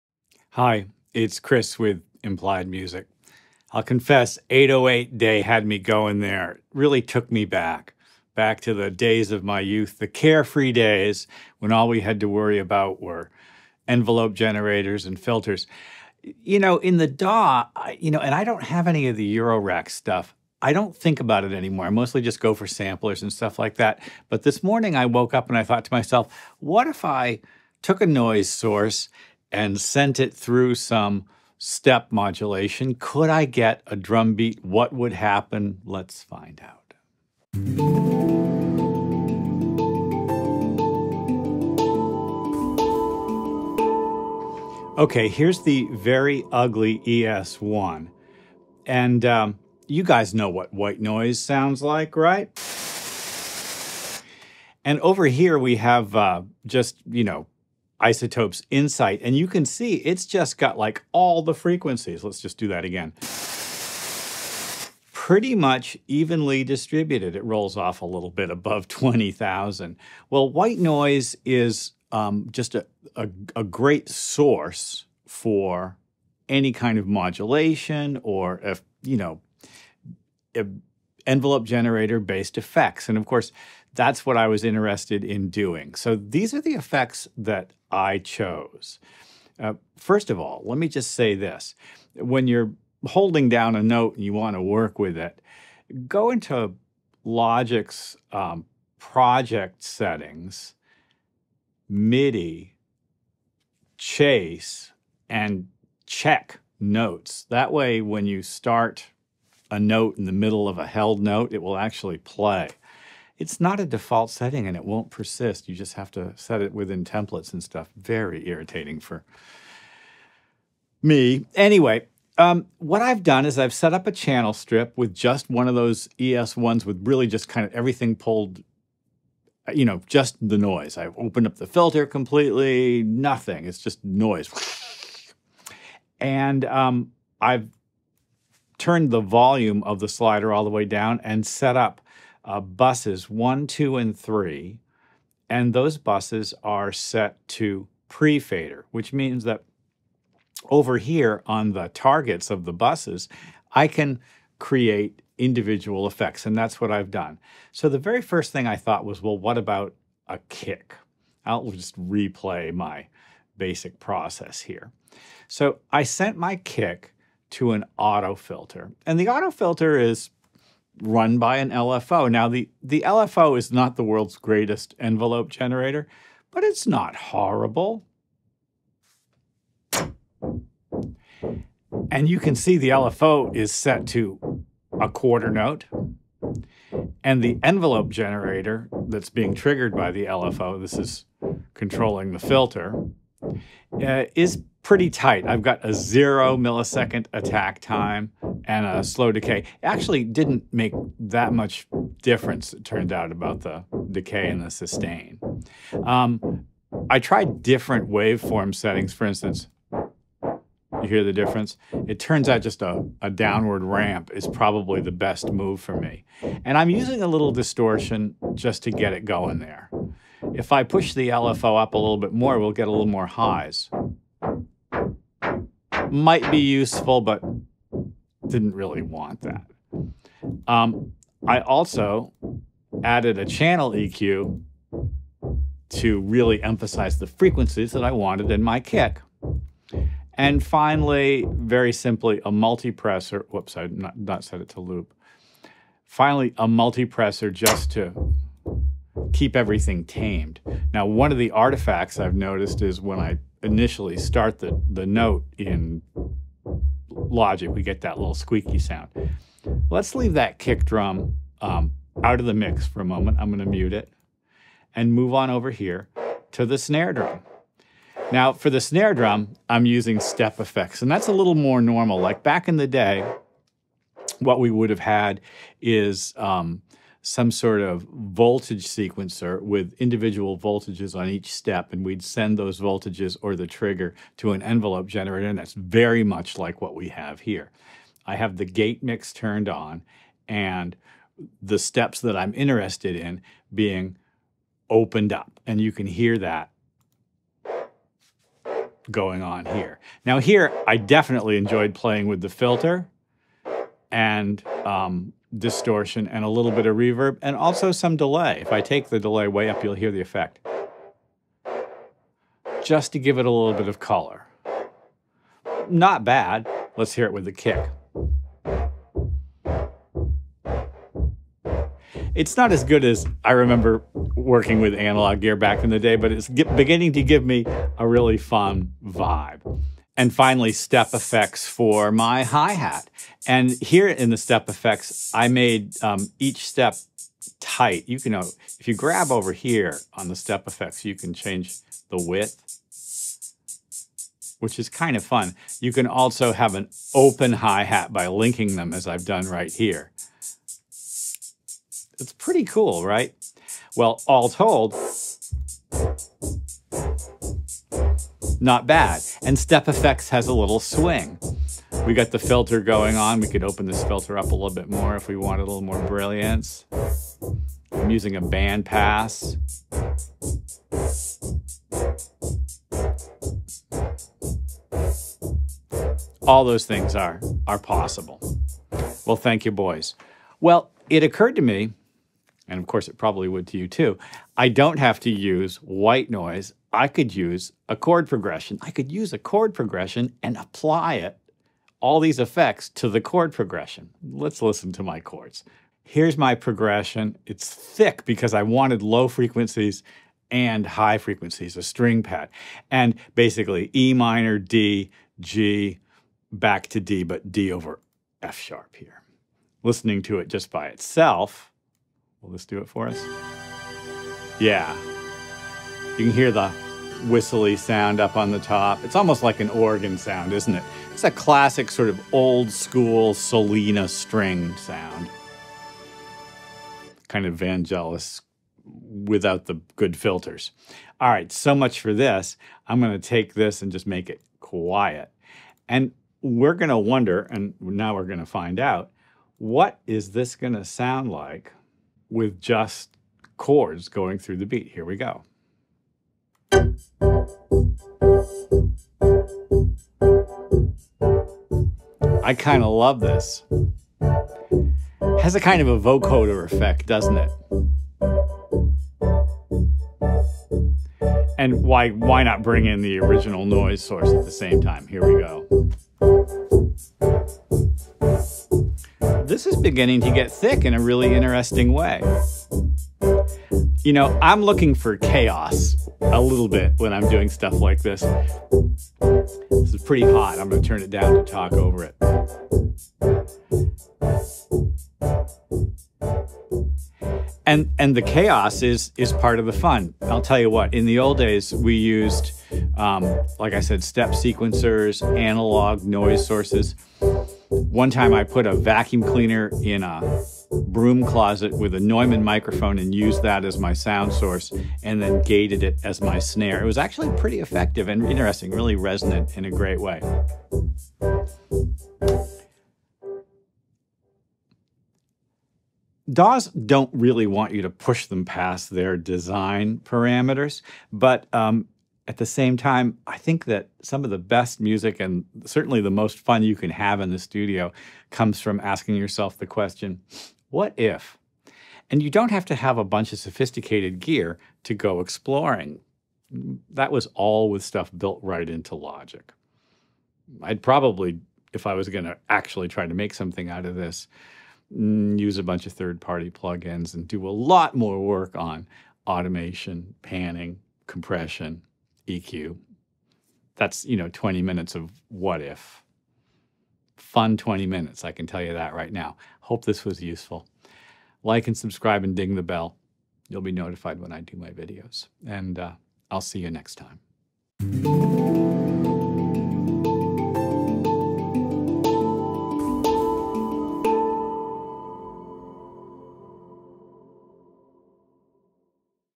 Step Modulation & LFO Autofilter tricks to turn Noise & Chords into Beats
White noise, Chords, clever processing with step FX and Filter Mods get me a beat!
00:00 intro
00:44 white noise and routing
02:38 Kick AutoFilter
04:25 Snare Step FX
05:30 HiHat Step FX
06:20 Chords instead of Noise
07:42 Together
08:07 conclusions